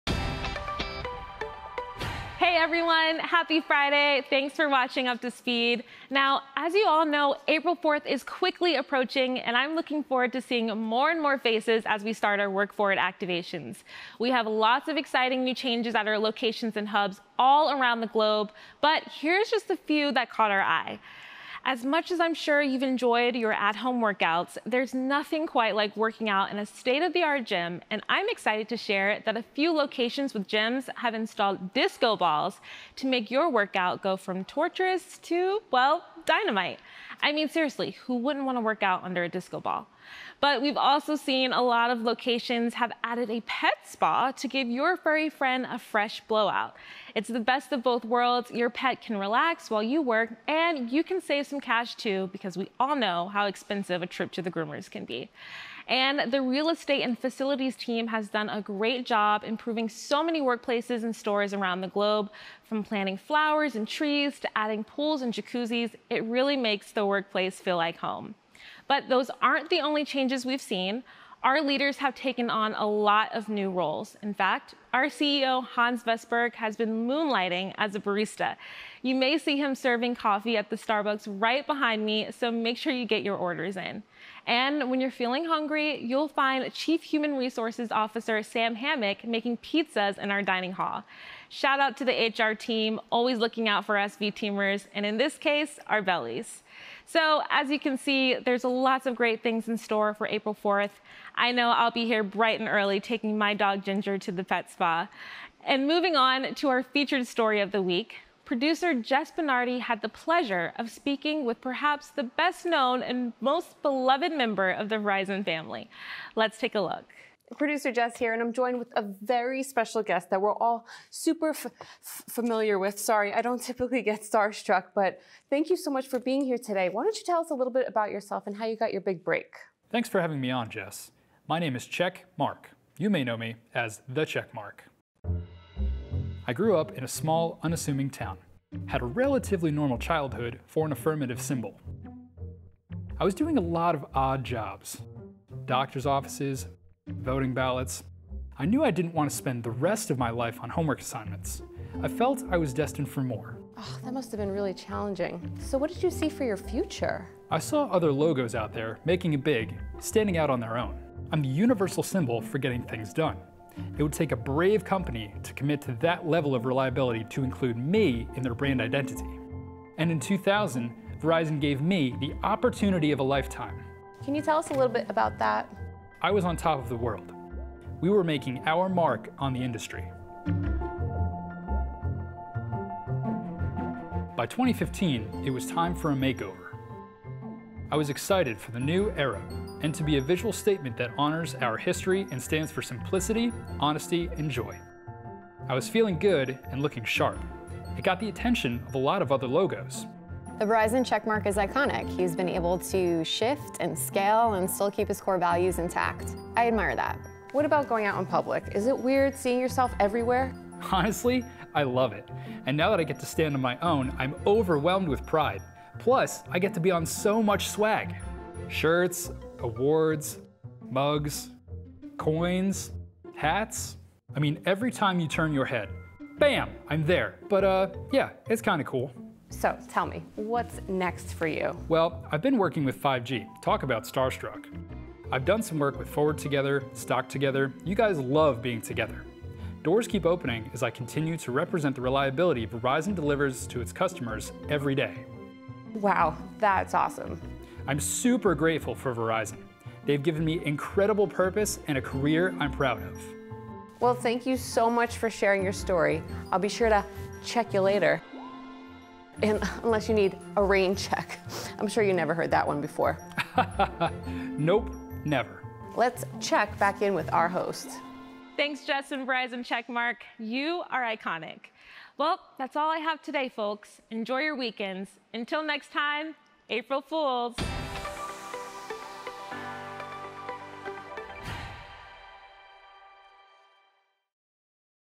Big Changes Happening Around The Verizon Office, Plus An Interview With A Literal Icon
Happy Friday! On today’s final Up To Speed of the week, host Raquel Wilson shares exciting updates happening around the office ahead of our Work Forward activations such as new facility additions and leadership position changes. AND Producer Jess Bonardi has an interview with the most iconic person at Verizon!